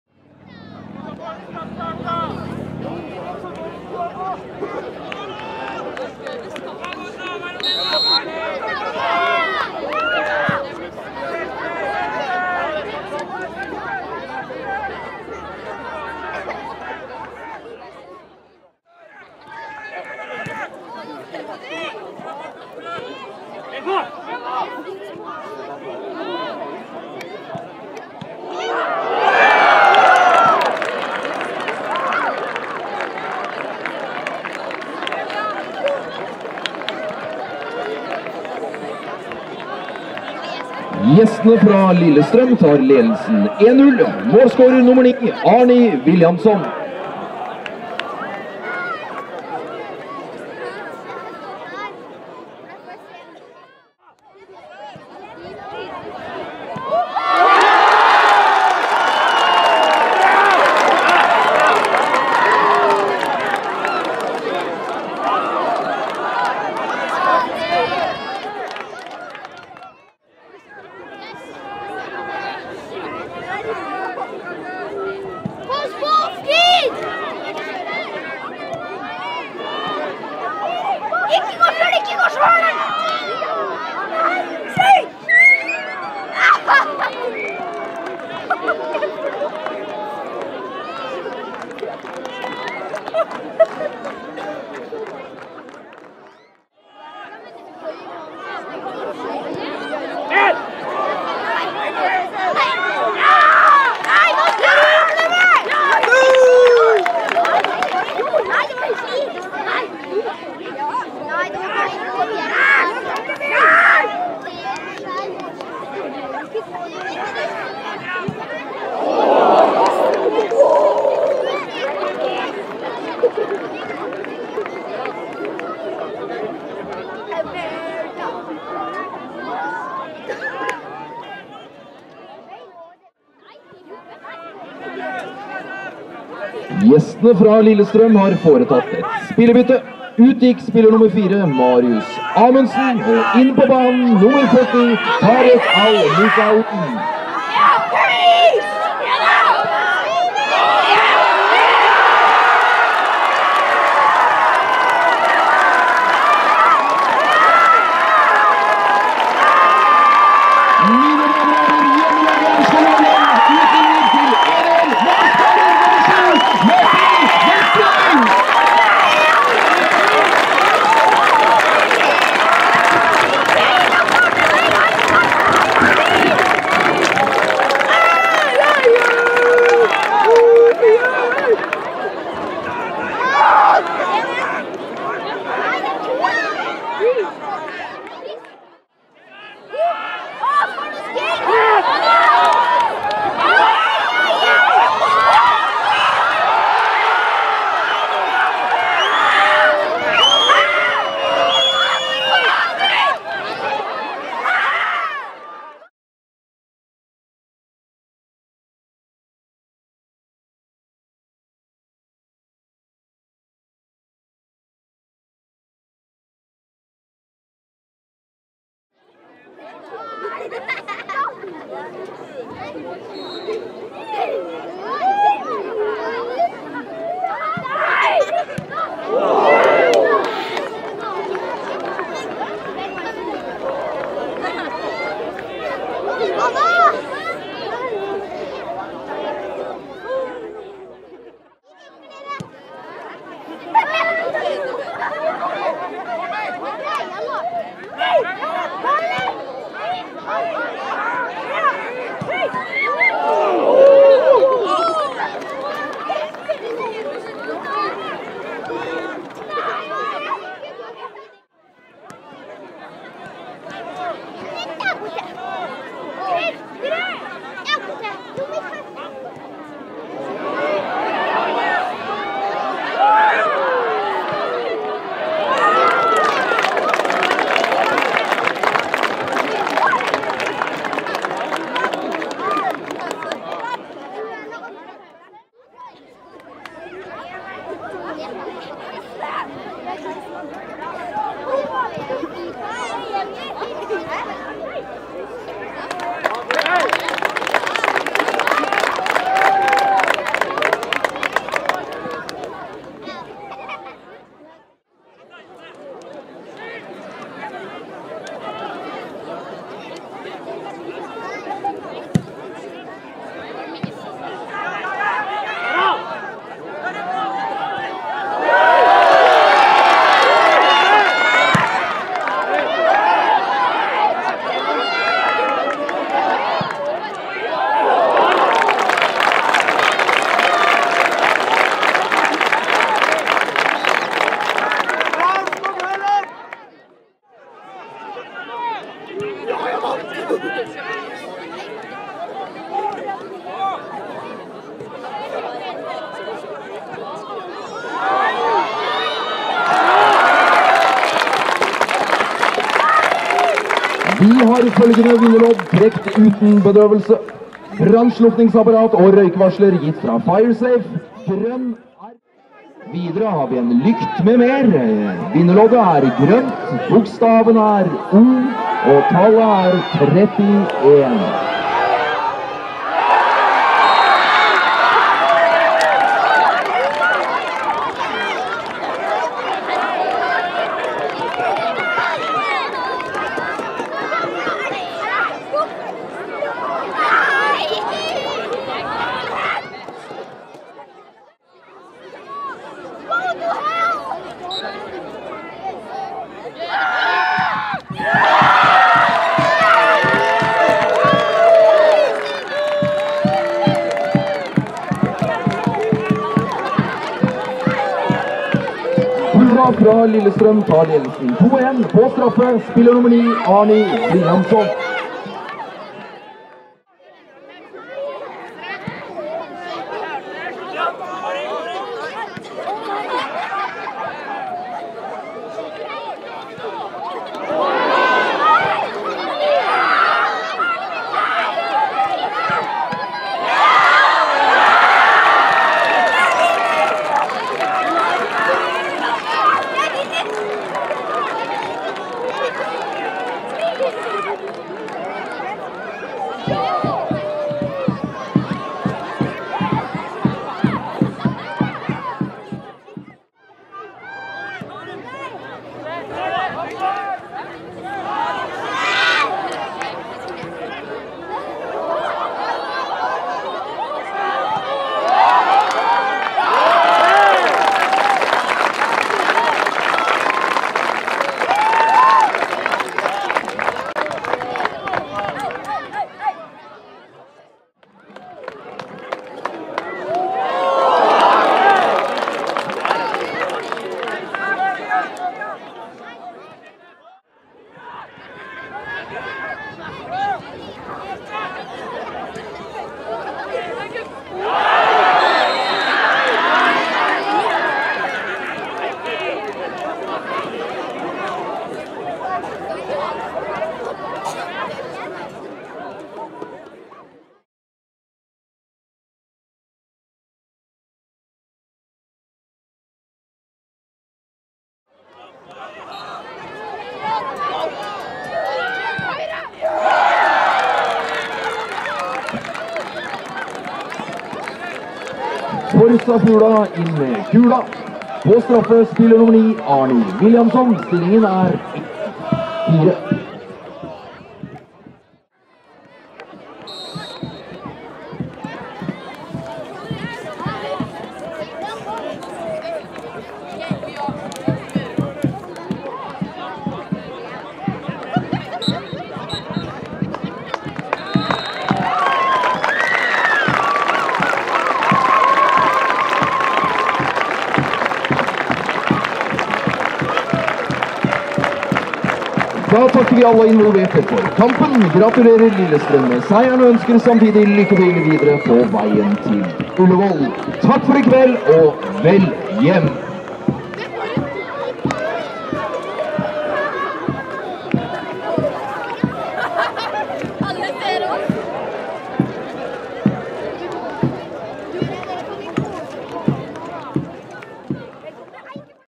Aurskog-Høland – Lillestrøm, sammendrag [2016-04-12, NM 2016, menn]
Kampinfo:
Lag [Nivå]: Aurskog-Høland [3.divisjon] – Lillestrøm [Tippeligaen]
Turnering: NM 2016 – 1. ordinære runde, menn
Dato: tirsdag 12. april 2016 kl. 18.00
Sted: Høland stadion

Kamptropp Aurskog-Høland (draktnummer i parentes):
Ahmed Jouini (12)
Bendik Vestreng (14)
Christian Hauger Helbostad (2)
Eirik Vedahl Haugen (6)
Fredrik Sether (15)
Hans Christian Bergsjø (4)
Henrik Jensen (9)
Henrik Studsrud (23)
Joar Akselsen (3)
Lars Kopperud (17)
Lars-Eirik Svardahl (11)
Marius Killingmo Kolstad (13)
Martin Fjellheim (8)
Martin Vestreng (7)
Mathias Nikolai Hoftvedt (1)
Steffen Sinkerud Krogstad (16)
Stian Skjeggenes (5)
Vegard Aaserud (20)

Kamptropp Lillestrøm (draktnummer i parentes):
Arnold Otieno (77)
Bassel Zakaria Jradi (7)
Bonke Innocent (18)
Frode Kippe (13)
Håkon Skogseid (16)
Imoh Fred Friday (23)
Jacob Rekdahl Faye Lund (12)
Jesper Flatla Kornbakk (32)
Jørgen Kolstad (17)
Malaury Jules Sacha Gabriel Martin (8)
Marius Ørjasæter Amundsen (4)
Martin Filip Falkeborn (2)
Mohamed Ofkir (20)
Ole Martin Rindarøy (5)
Sheriff Sinyan (26)
Simen Kind Mikalsen (3)
Tarik al Moussaoui (40)
Árni Vilhjálmsson (9)

Det norske seriesystemet i fotball for menn (pr. 2016): Tippeligaen, OBOS-ligaen (1.divisjon), PostNord-ligaen (2.divisjon), Regionsligaen (3.divisjon), 4.-9.divisjon